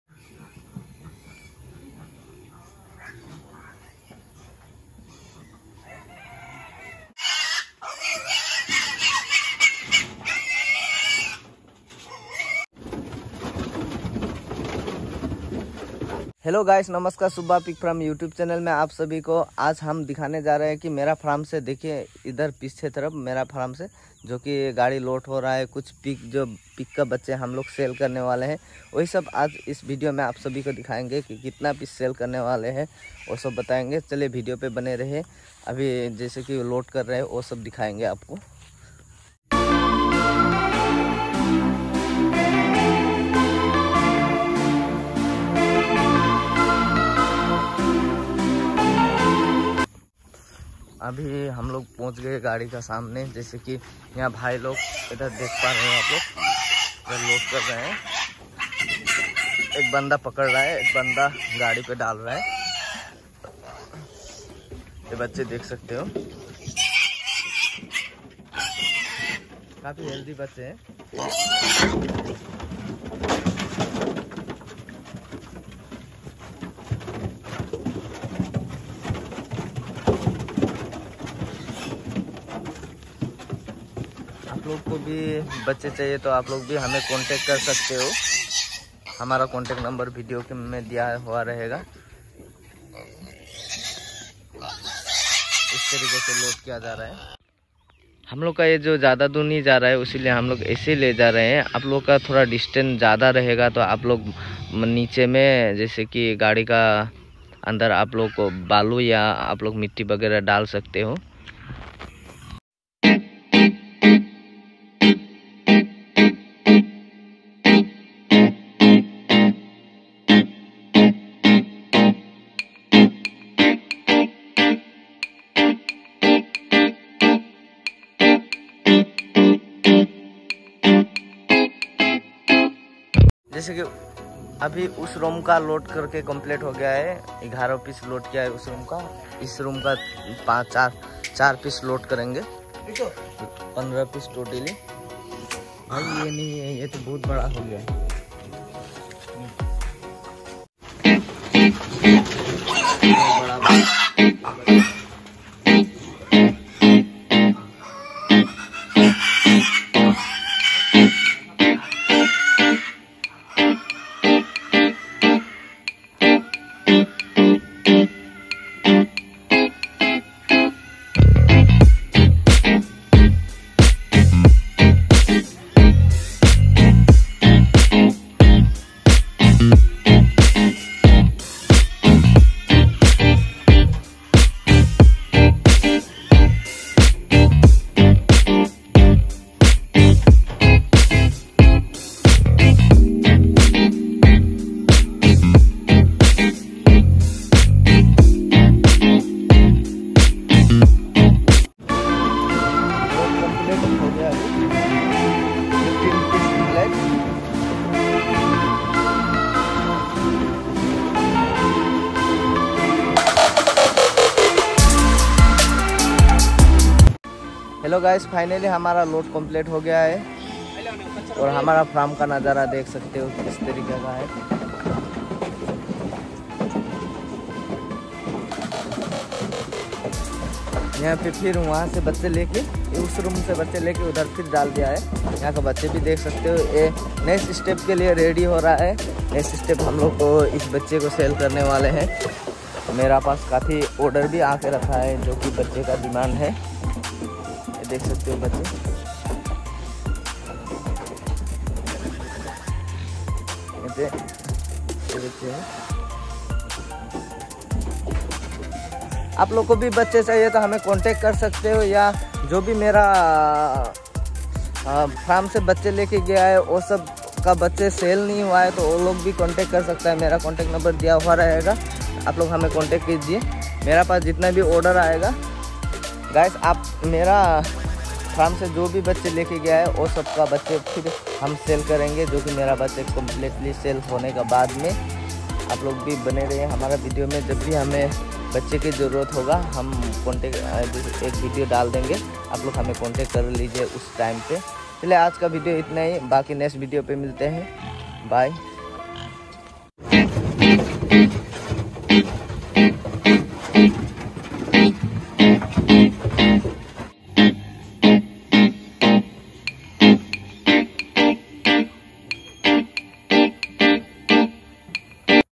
आज 15 सूअर के बच्चे बिके// सुअर पालन//सुब्बा सुअर फार्म (असम)
Hello guys welcome to my Subba pig farming (ASSAM)￼

Today 15 piglet sell